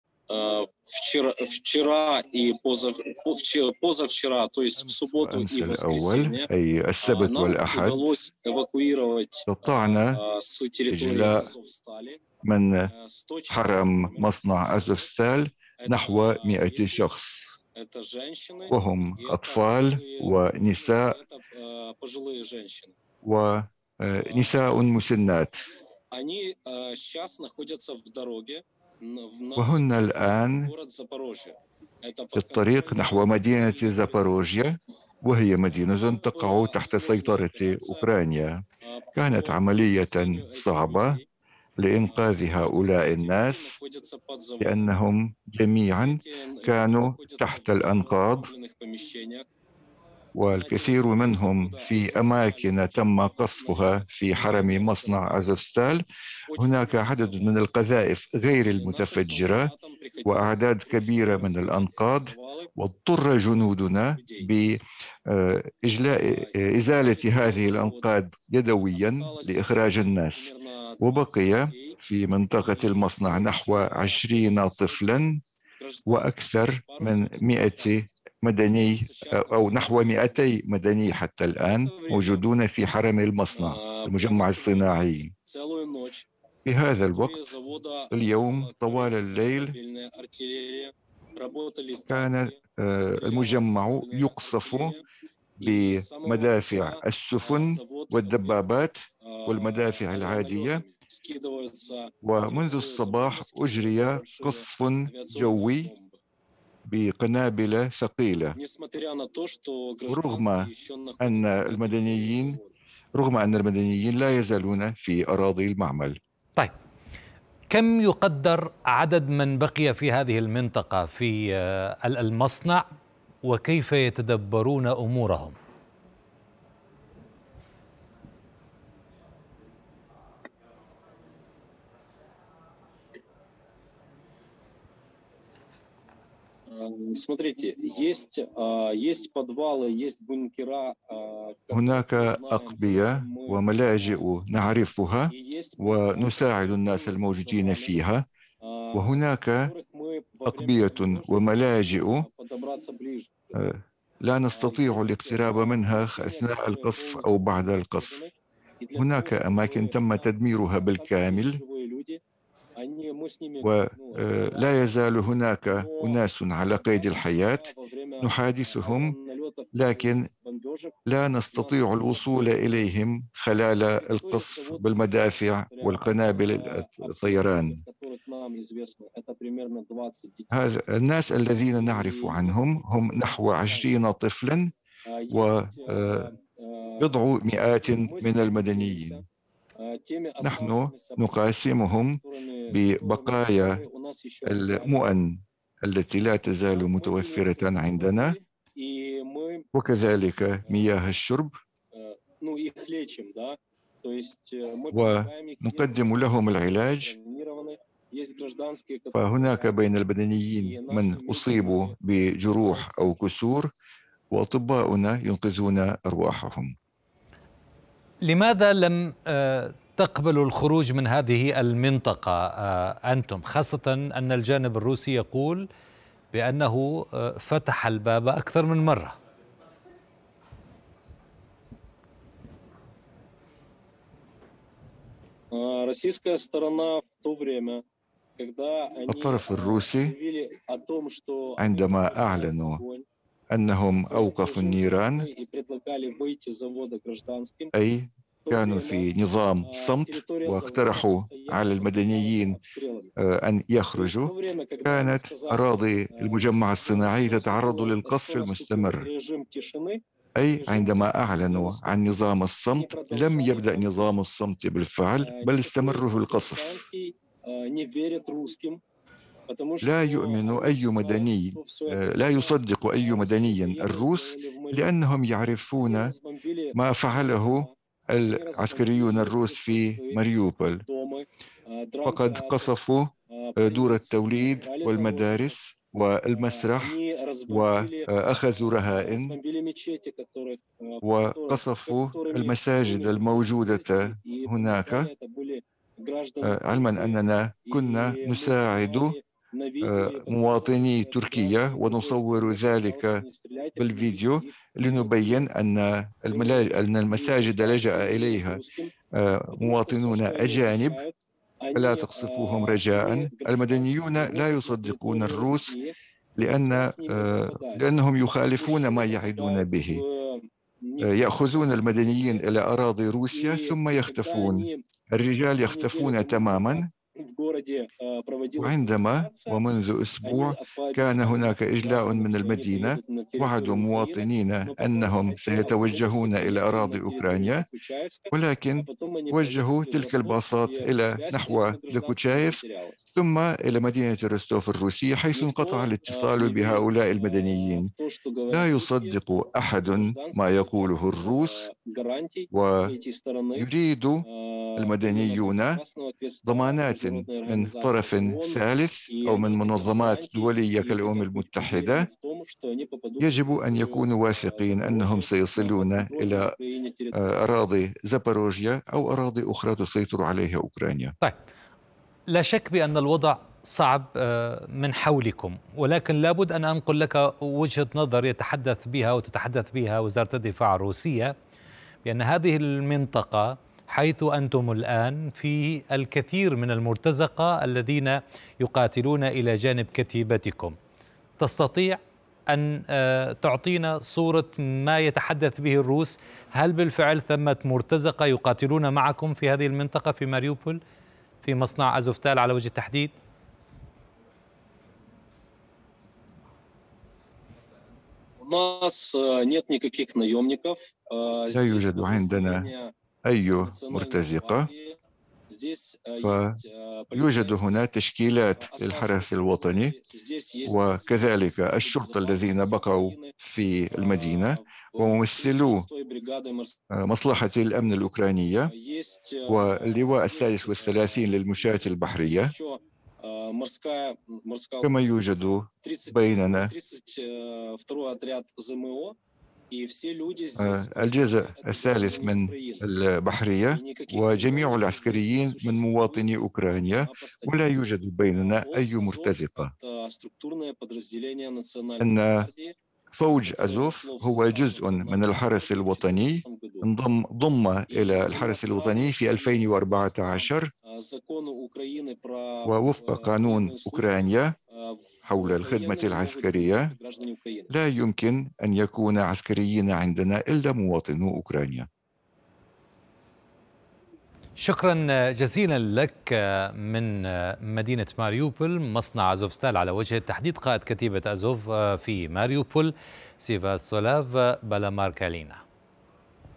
لماذا يرفض مقاتلو آزوف الخروج من ماريوبول؟ حوار مع قائد الكتيبة الأوكرانية
لماذا يرفض مقاتلو كتيبة آزوف الخروج من مصنع آزوفستال وهل يوجد "مرتزقة" يقاتلون إلى جانبهم وماهي مخاوف المدنيين هناك.. هذه الأسئلة وغيرها طرحناها على "سفياتوسلاف بالاماركالينا" القائد في كتيبة آزوف